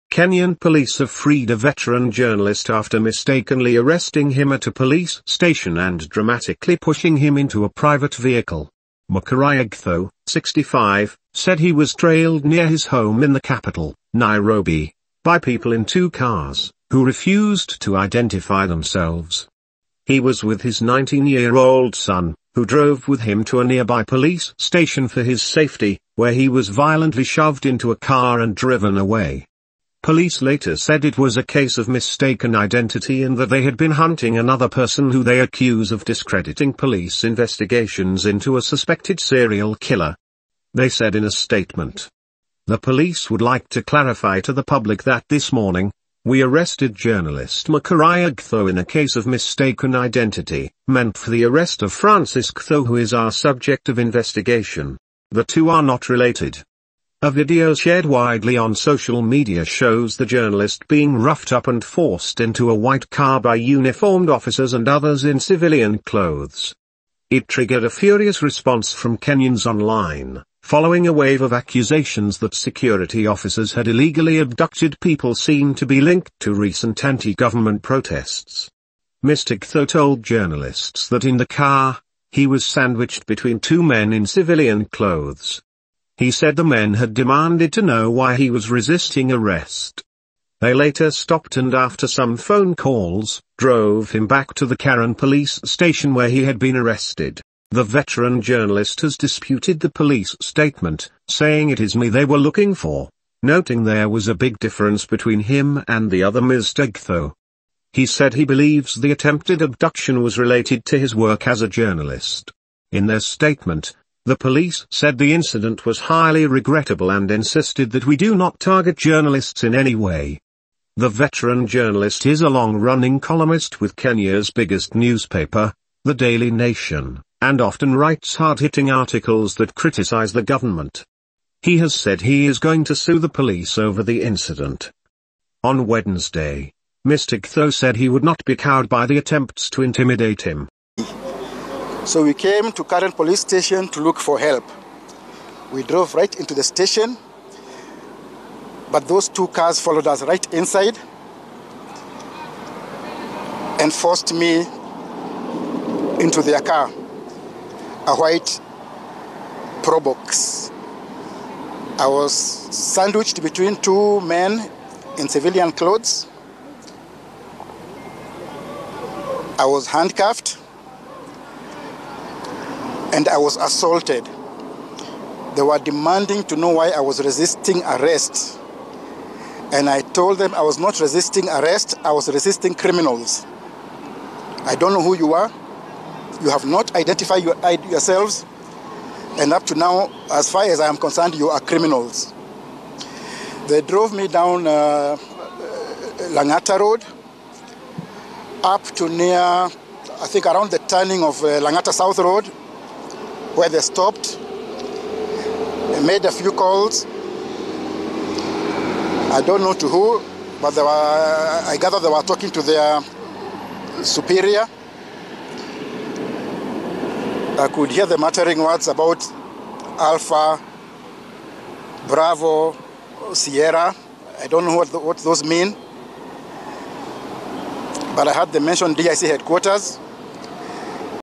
Macharia Gaitho Abduction: Kenyan journalist freed after 'mistaken' arrest drama– News54 Africa
Kenyan police have freed a veteran journalist after “mistakenly” arresting him at a police station and dramatically pushing him into a private vehicle.
Macharia Gaitho, 65, said he was trailed near his home in the capital, Nairobi, by people in two cars, who refused to identify themselves.